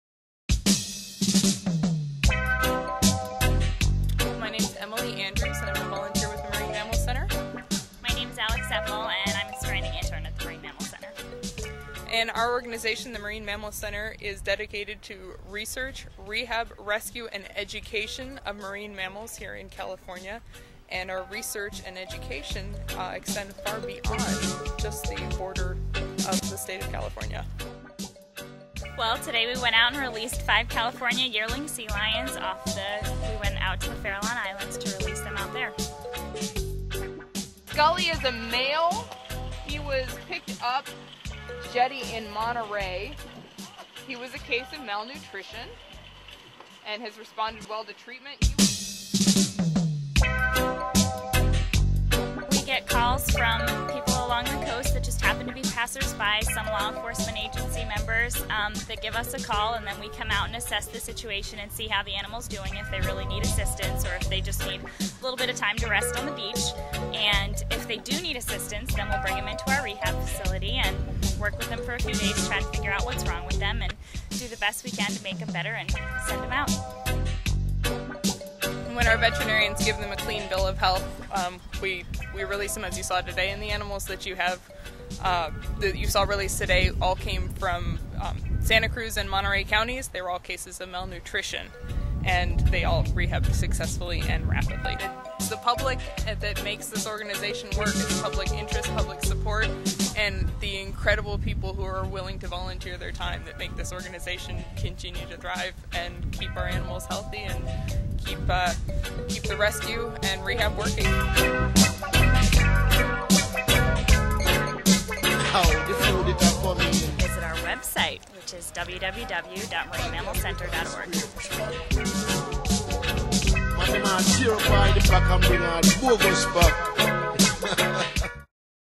Sea Lions of California Marine Sanctuaries
The Marine Mammal Center of Sausalito CA is dedicated to rehabiliate stranded, ill or injured marine mamals and educate the public on the importance of these enigmatic sea creatures. We follow the release of several rehabilitated sea lions and learn from MMC volunteers.